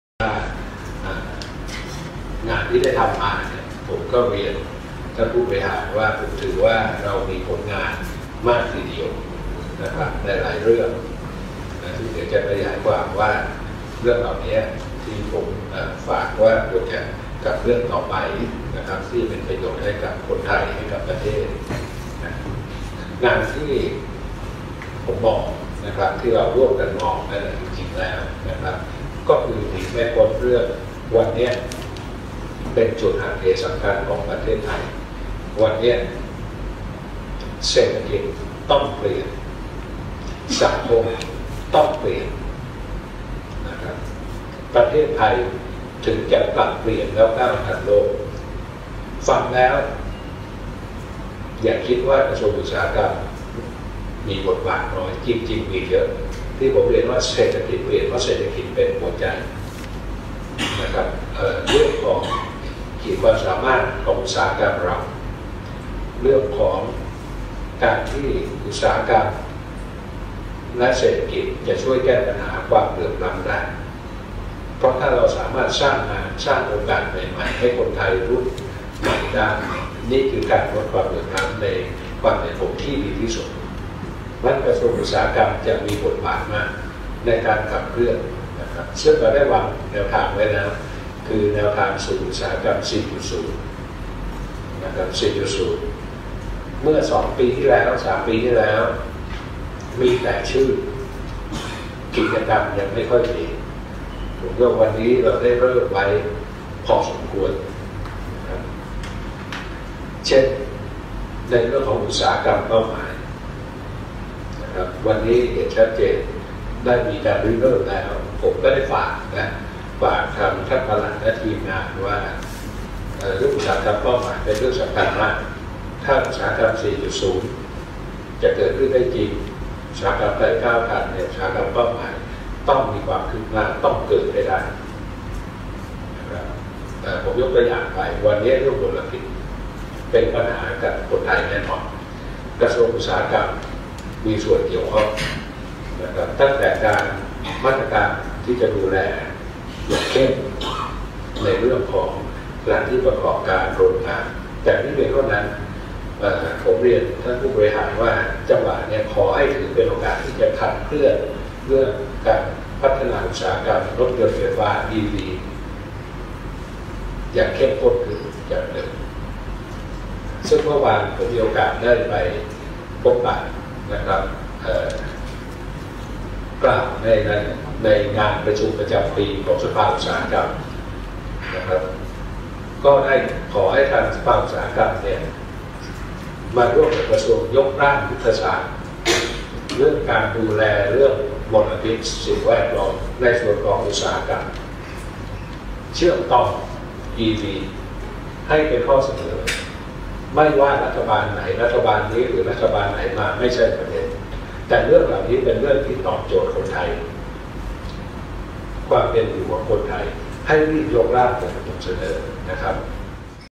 “อุตตม” อำลาผู้บริหาร ก.อุตฯ ฝากขับเคลื่อน ศก.เปลี่ยนประเทศ วอนอย่าใส่เกียร์ว่าง
29/01/2019 “อุตตม” อำลาผู้บริหาร ก.อุตฯ ฝากขับเคลื่อน ศก.เปลี่ยนประเทศ วอนอย่าใส่เกียร์ว่าง
“อุตตม” อำลาผู้บริหารกระทรวงอุตสาหกรรมฝากงานให้สานต่อเพียบโดยเฉพาะการขับเคลื่อนอุตสาหกรรม 4.0 เพื่อปรับเปลี่ยนประเทศไทย ย้ำบทบาทก.อุตฯสำคัญมาก ขอให้ดัน 2 อุตฯเป้าหมายทั้งอุตสาหกรรมเศรษฐกิจชีวภาพ ศก.หมุนเวียน ขับเคลื่อนอีอีซี วงในเผย”อุตตม”วอนให้ราชการอย่าใส่เกียร์ว่างระหว่างนี้